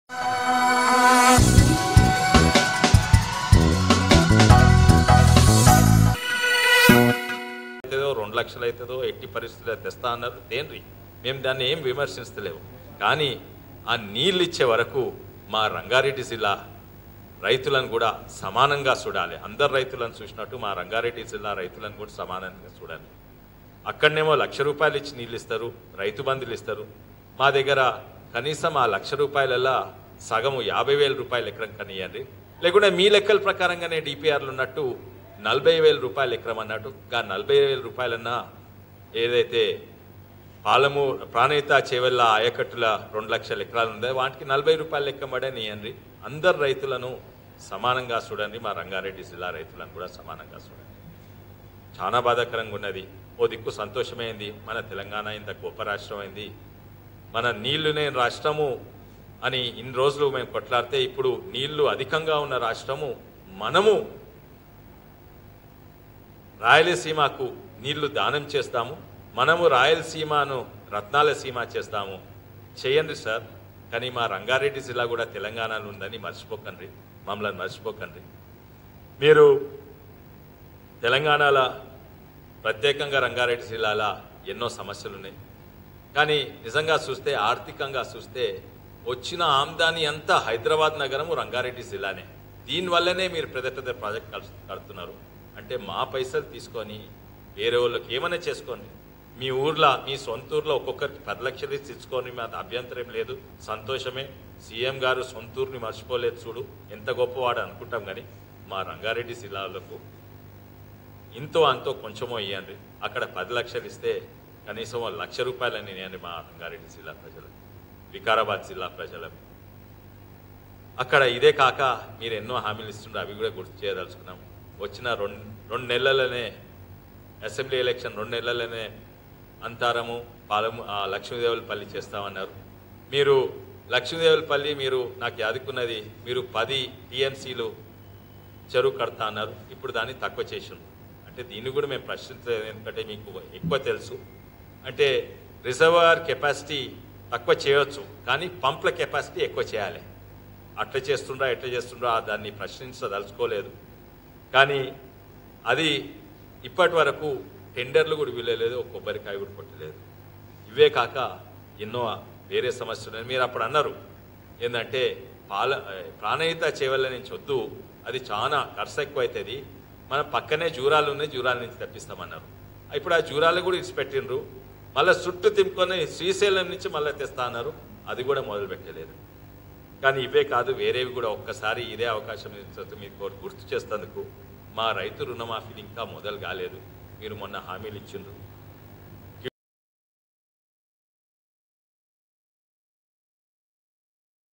Congress Leader Konda Vishweshwar Reddy Press Briefing On Water Issue in Rangan Reddy| Overseas News
Congress Leader Konda Vishweshwar Reddy Press Briefing Over Water Issue in Rangan Reddy